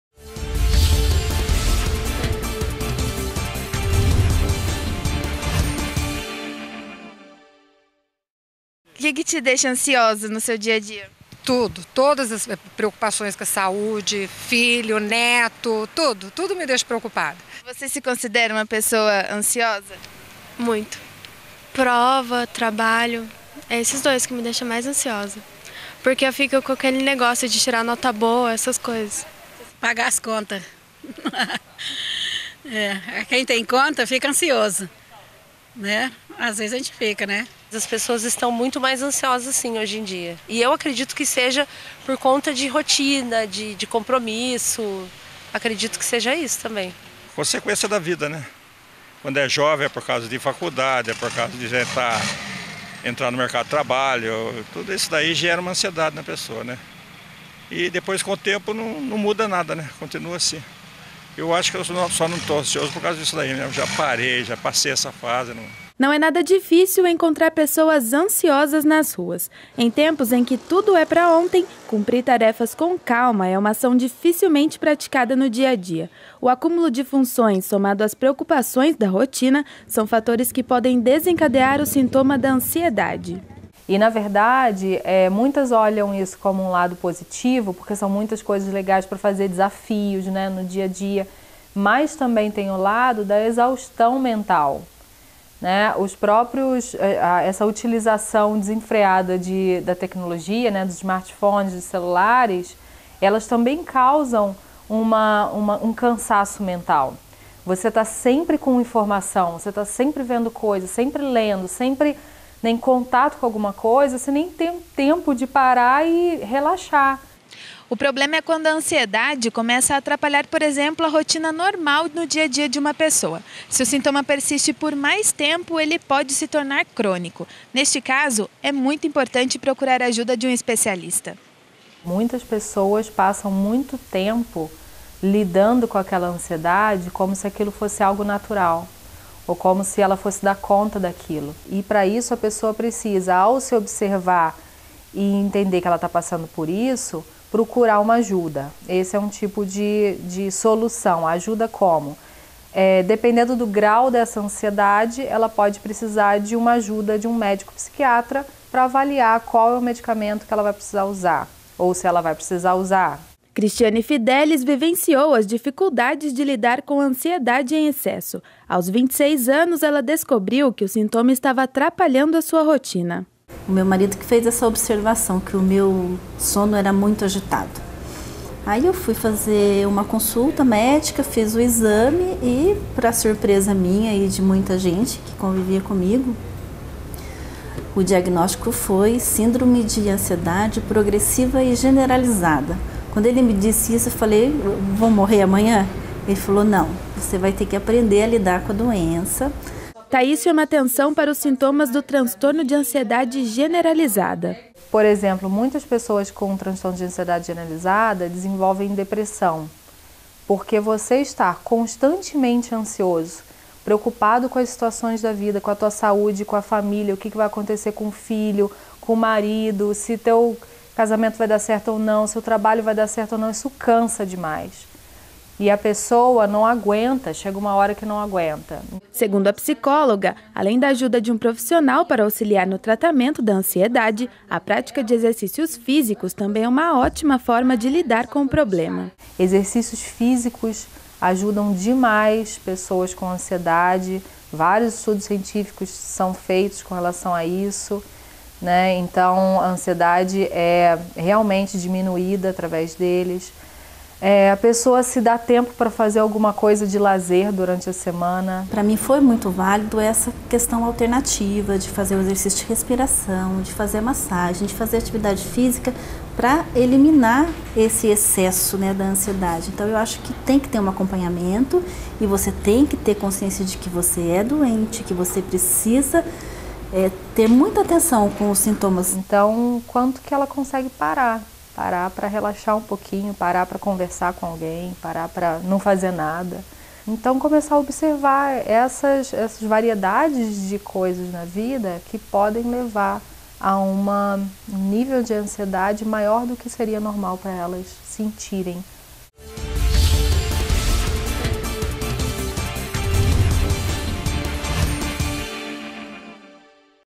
Transtorno - Ansiedade
Saiba mais sobre a ansiedade, doença que atinge milhões de brasileiros.